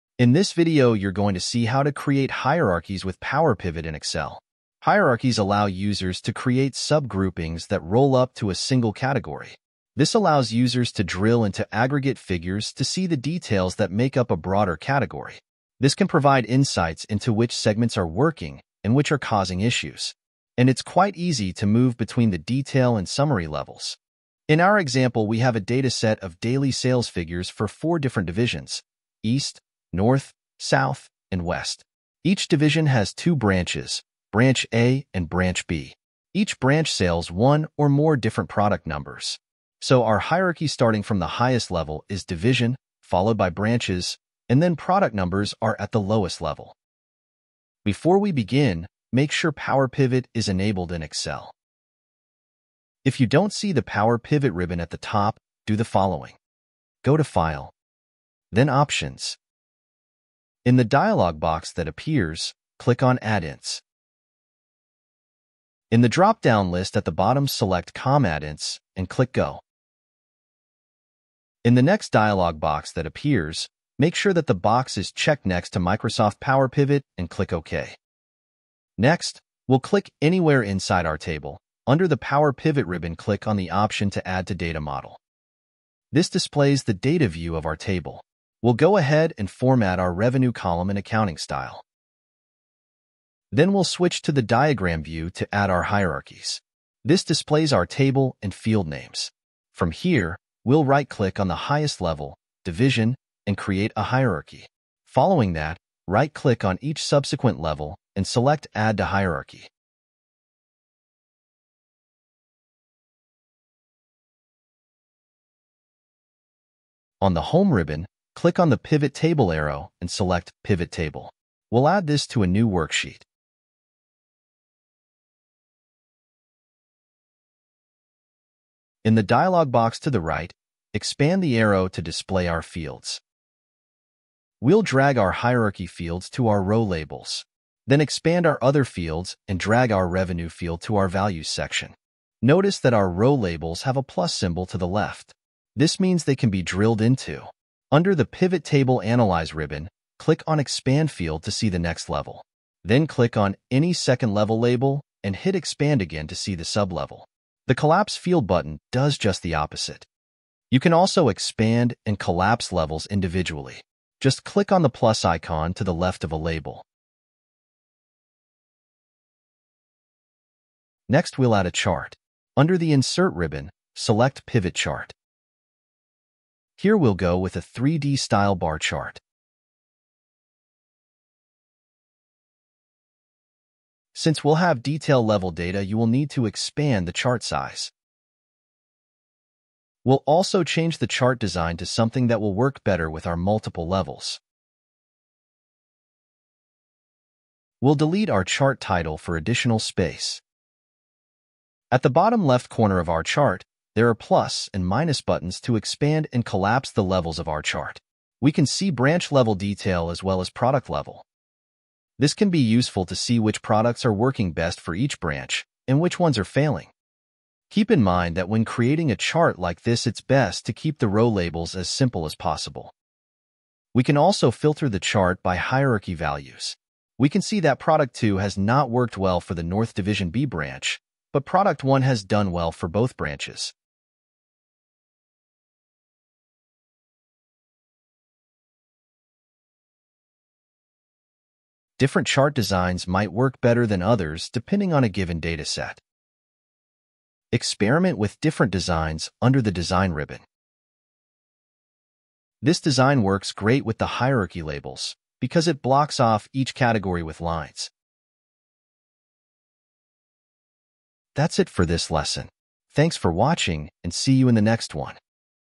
Excel Power Pivot Hierarchies
This video illustrates how to create Hierarchies in Power Pivot with Excel.  Power Pivot Hierarchies allow users to quickly drill into detail level data on Pivot Tables and Charts.  This can help end users see what's behind the summary level figures.